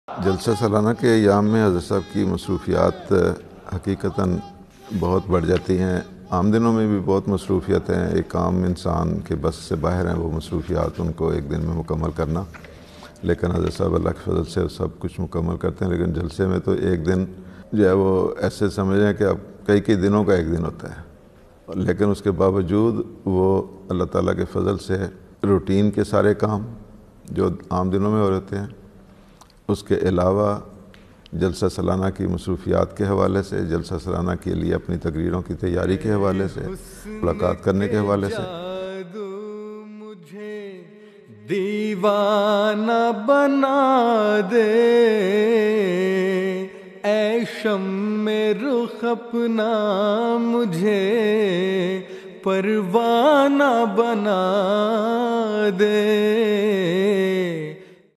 Muslim Leader Ahmadiyya
Ahmadiyya Muslim leader and Imam Hazrat Mirza Masroor Ahmad with English subtitles